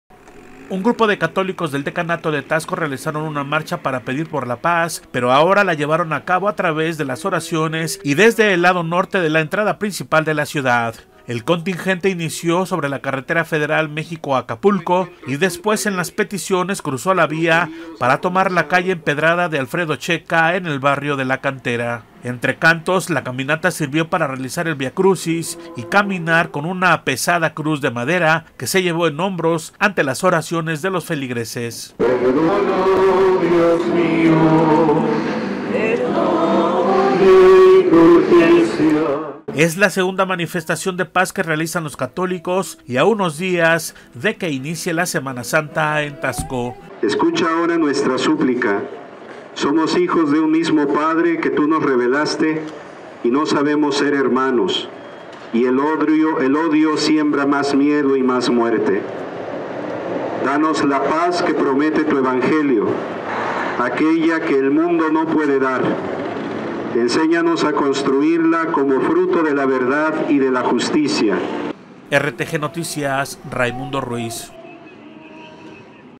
RTGnoticias | Realizan segunda marcha por la paz en Taxco
Un grupo de católicos del decanato de Taxco, realizaron una marcha para pedir por la paz, pero ahora la llevaron a cabo a través de las oraciones y desde el lado Norte de la entrada principal de la ciudad.